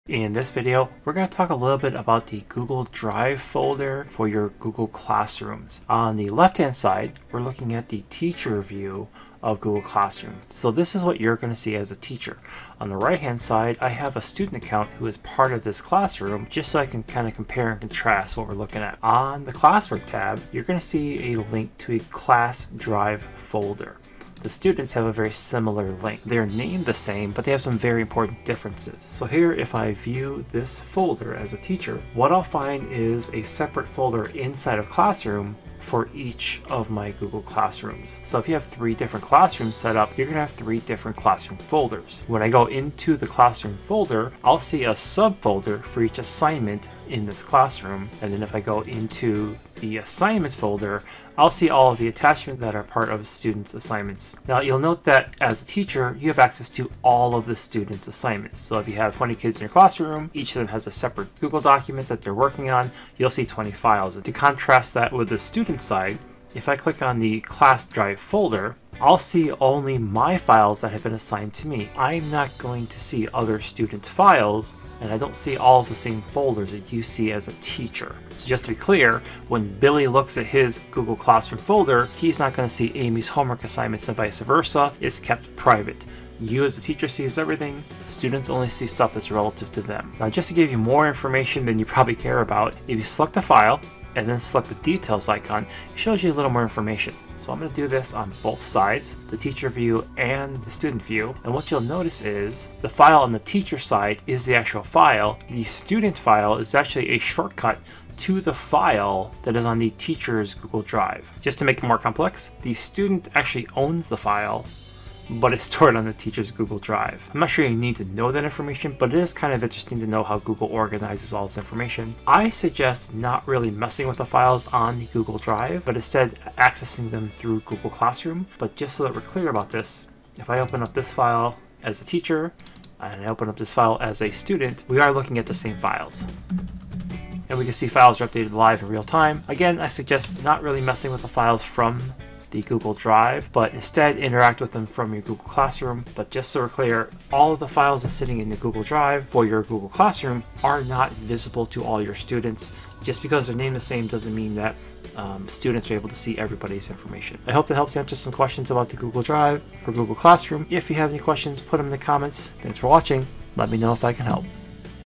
Class Folder in Google Drive
Learn about how file in your Google Classroom are stored in Google Drive.  Compare what teachers see vs. the student view.  Dive into the details about who owns the files and how student work is kept separate.

🎞️ I use Camtasia to record my videos.  Try Camtasia for free

© Bryant Cobarrubias (@bryantcobo)